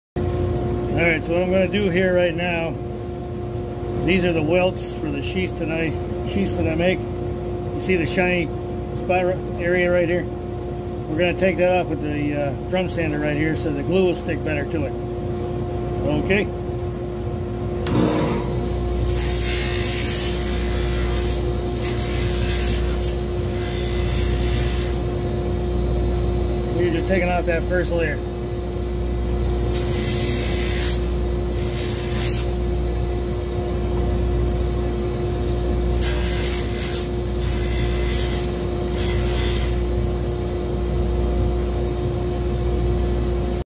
Leather Working, Sanding The Welts On A Drum Sander
Sanding off the shiny side of these leather welts for knife sheaths so the glue sticks better.

And if you would like to be notified by email when we have new knives available to purchase please join our waitlist

Scott Hawley